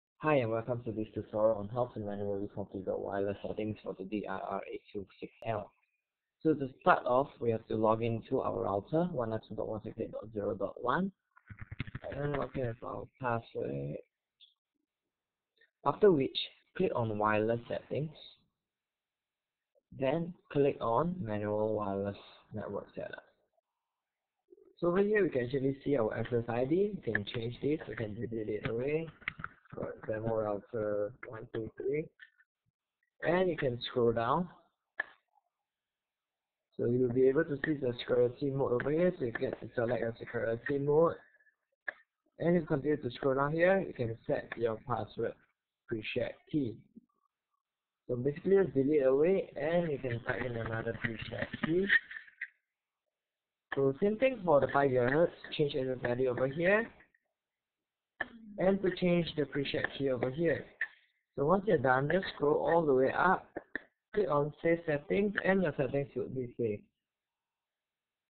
D-link Router How-To: How to do Manual Wireless Setting
Watch this tutorial on How to Manually Configure Wireless Setting your router

Supported Models: DIR-636L, DIR-826L, DIR-820L

Is this video useful to you? Would you like to see more of such video? We love to hear from you.